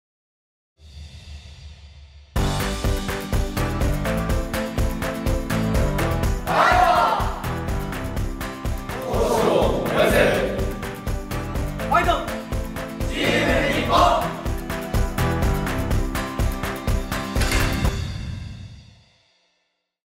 19WKCカウントダウン ミラノへの道（あと30日）
第19回世界剣道選手権大会まであと30日となりました。第19回世界剣道選手権大会までのカウントダウン動画を投稿します。みんなで日本代表を応援して、世界剣道を盛り上げましょう！

第19回世界剣道選手権大会
期日：2024年7月4日～7月7日
開催地：イタリア ミラノ